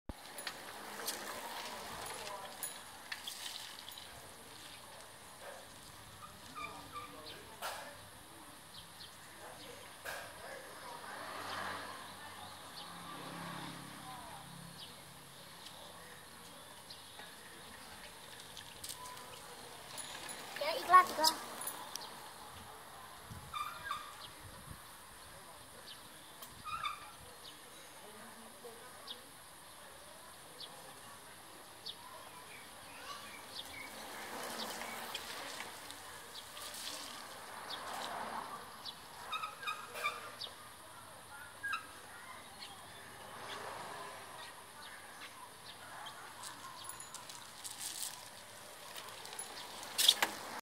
นโม ปั่นจักรยาน 2 ล้ออย่างซิ่ง
นโมอายุ 5 ขวบ 10 เดือน ปั่นจักรยานในวันฝนตกพรำๆ
วันอังคาร ที่ 12 พฤษภาคม 2563
เว้นระยะห่างทางสังคม โควิด-19